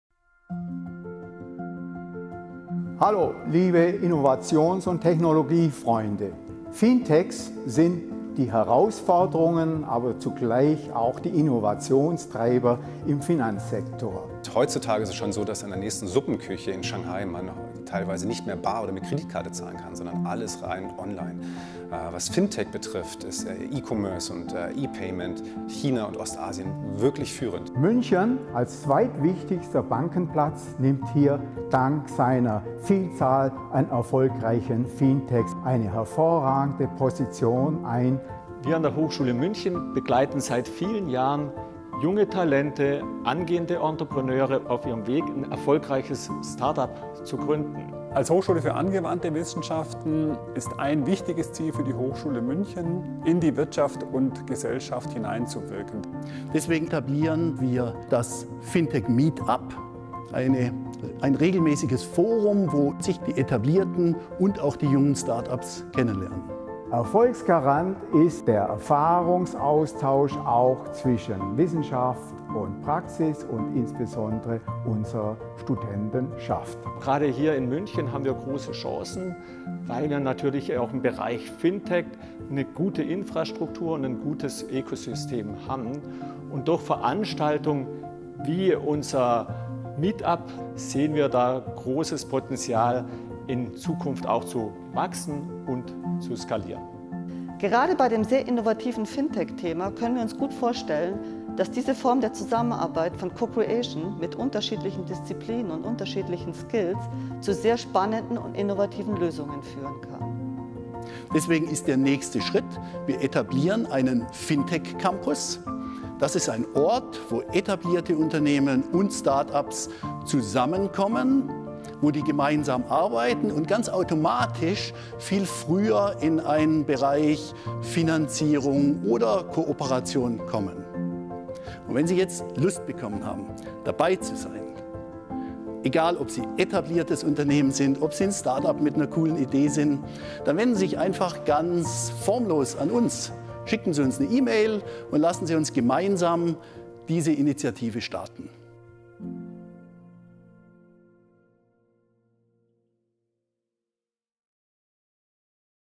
FinTech am SCE und der Hochschule München (HM)
München ist im Ranking der Finanzplätze in Deutschland direkt nach Frankfurt ganz vorne auf Nummer zwei. München ist wirtschaftlich stark und als Landeshauptstadt Bayerns bedeutsam. Dennoch gibt es abgesehen von einigen wenigen Initiativen kein wirkliches Forum für Finanztechnologie. Die Hochschule München und ihr Entrepreneurship-Center, das Strascheg Center for Entrepreneurship möchten das ändern.
Bei den ersten erfolgreichen FinTech MeetUps 2018 bis 2020 zeigte sich ein großer Bedarf für die Vernetzung von Start-ups und etablierten Unternehmen im Bereich FinTech an einem Ort. Deshalb bauen wir nun in wechselnden Formaten ein Ecosystem im Bereich FinTech auf, das es Unternehmen der Finanzindustrie und innovativen FinTech Startups erlaubt, schneller zu Kooperationen zu kommen.

Sehen Sie dabei und melden Sie sich!
Strascheg Center for Entrepreneurship (SCE) der Hochschule München
Michael Hack